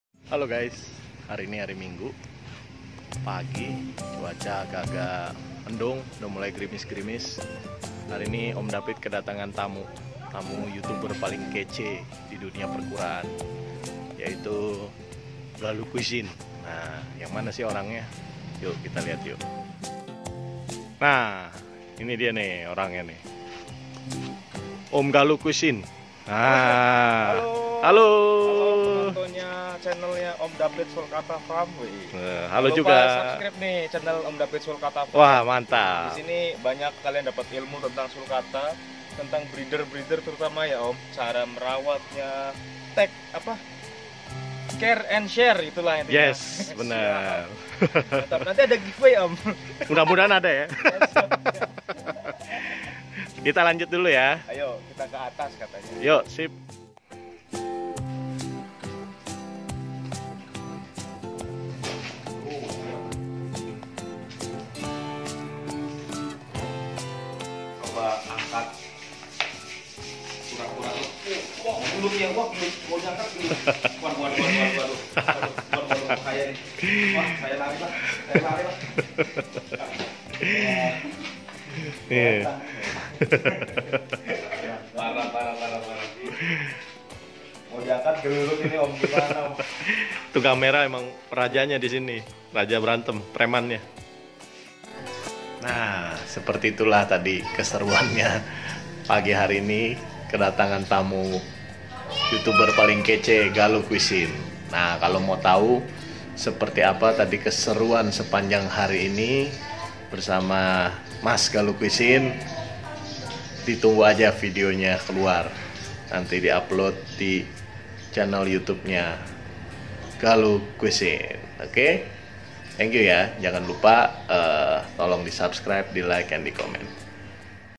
OmDapit : Kedatangan Youtuber hits "GALUH CUISINE"
Di video ini om dapit kedatangan tamu spesial dari tegal, youtuber hits dengan subscriber yang berjumlah ratusan ribu... GALUH CUISINE... Check it out...